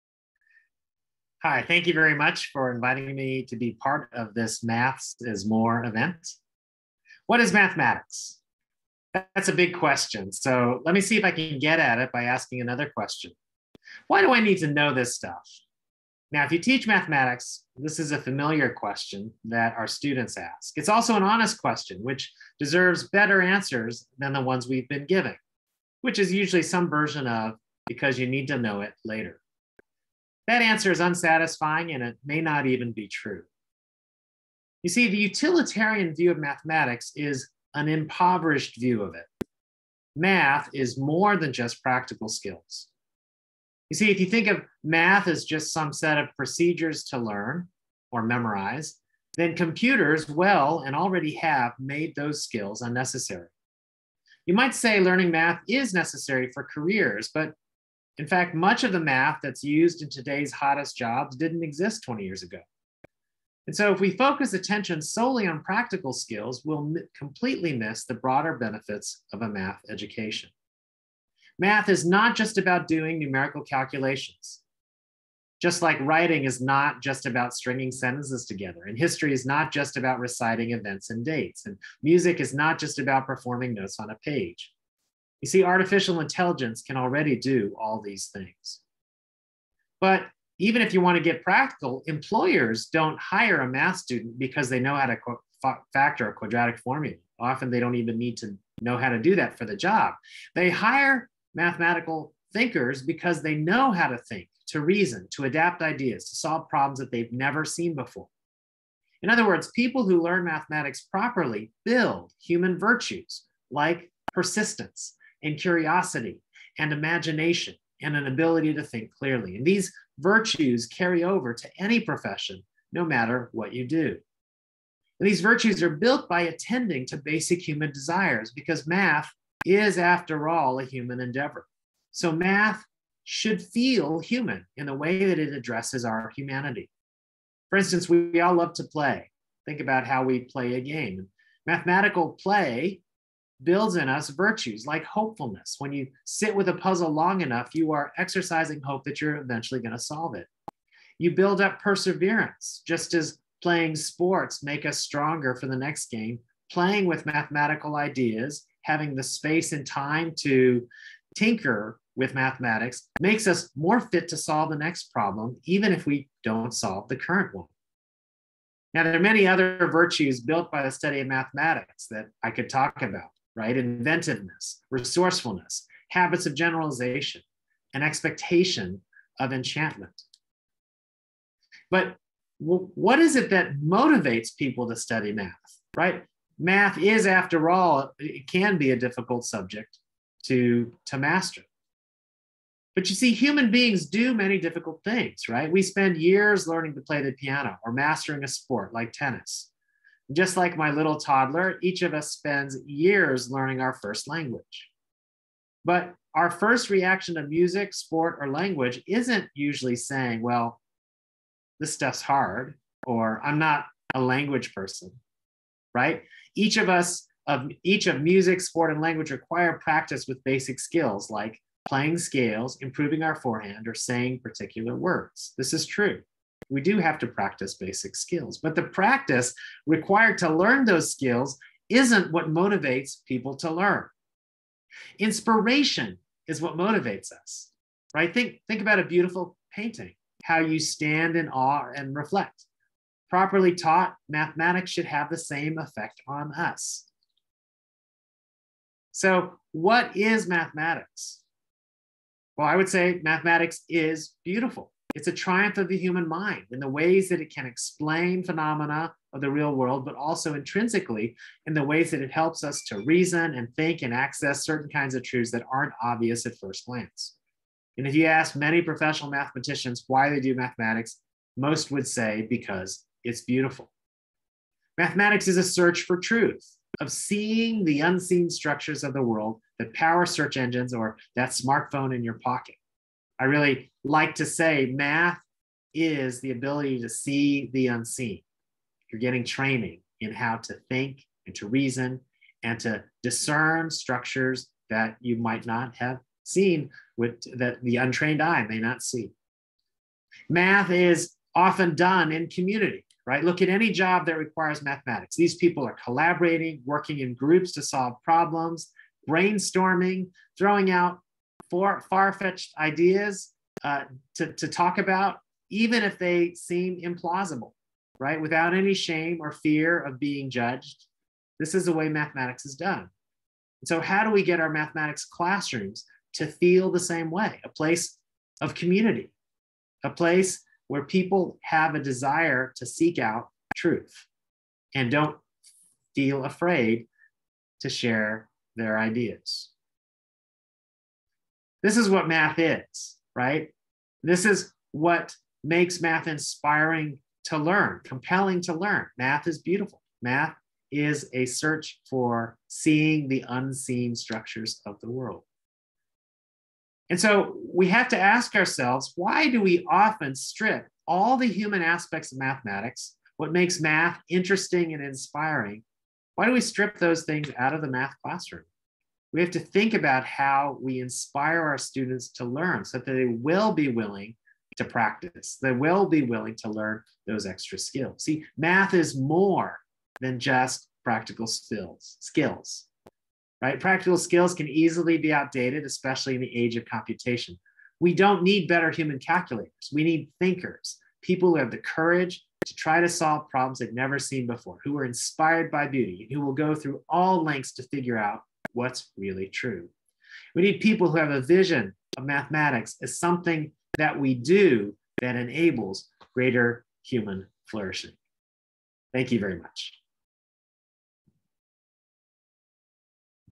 Maths is more talk by Prof Francis Su
A short talk on the theme of 'Maths is more' by Professor Francis Su.